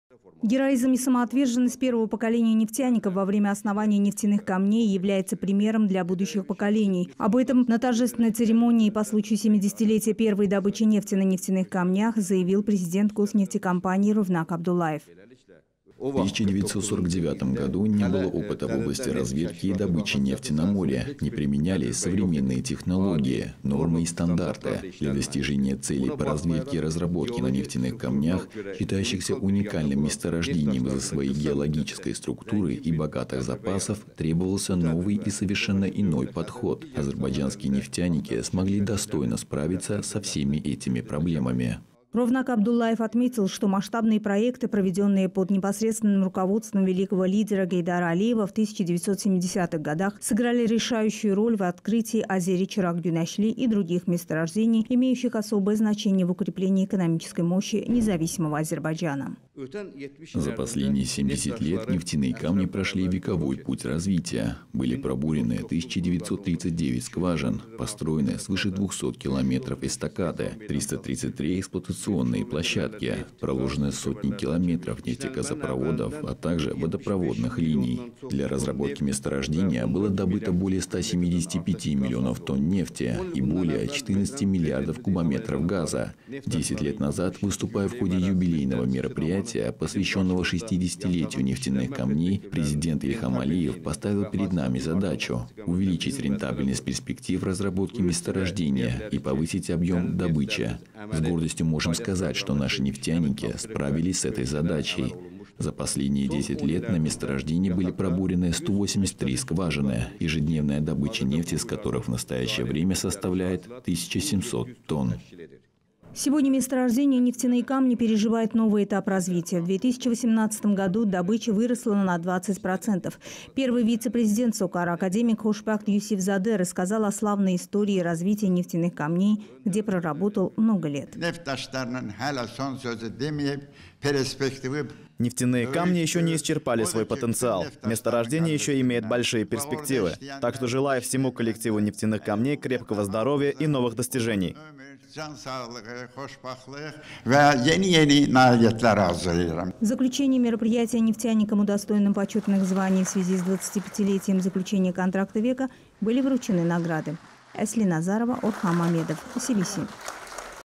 В головном офисе SOCAR отметили юбилей первой в мире нефтяной скважины в открытом море
По случаю 70-летия первой добычи нефти на «Нефтяных Камнях» руководство и сотрудники Госнефтекомпании Азербайджана SOCAR посетили могилу и почтили память общенационального лидера азербайджанского народа, архитектора независимого азербайджанского государства, сыгравшего исключительную роль в развитии экономики страны, основателя новой нефтяной стратегии Гейдара Алиева.

Затем в головном офисе SOCAR состоялась торжественная церемония. Президент Госнефтекомпании Ровнаг Абдуллаев отметил, что героизм и самоотверженность первого поколения нефтяников во время основания «Нефтяных Камней» является примером для будущих поколений.